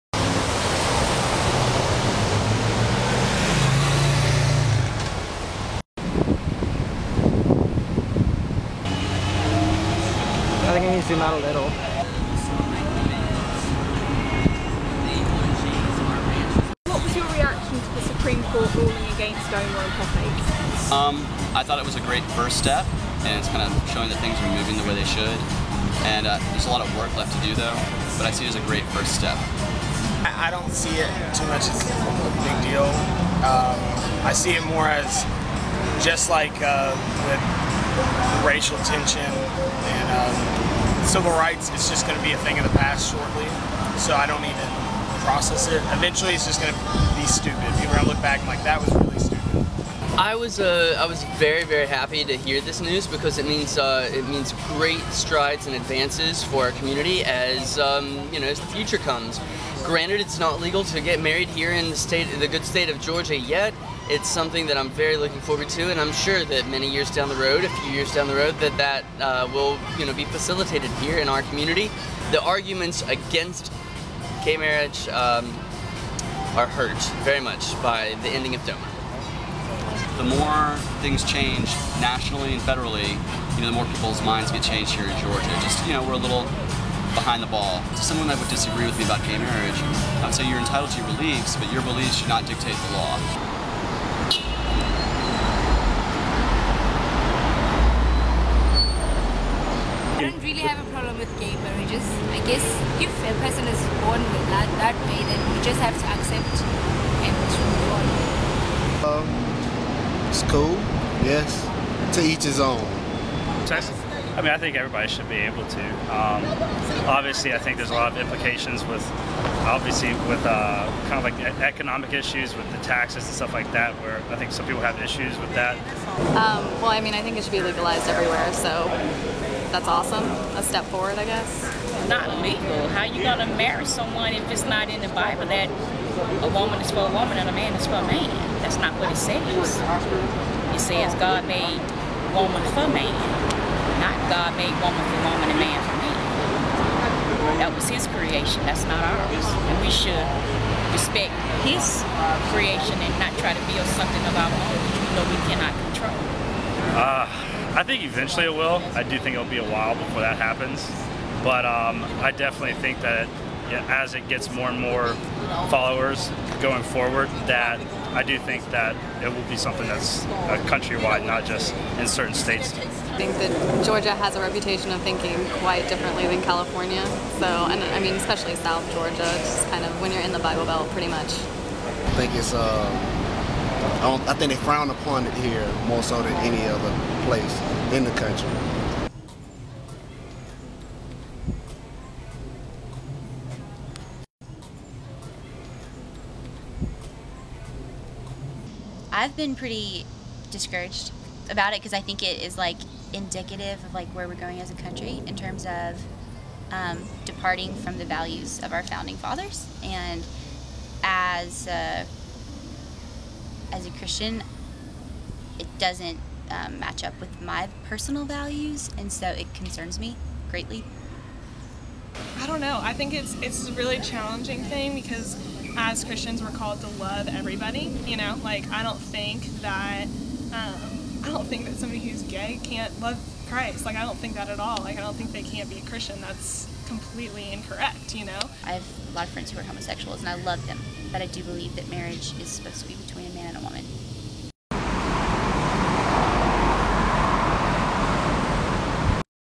Atlanta's Opinion on LGBT Rights
We went out and asked the people of Atlanta, their opinions on the overruling of DOMA, gay marriage and gay rights in general. Thanks to everyone who contributed, especially Joe's On Juniper.

Vox Media Cafe 2013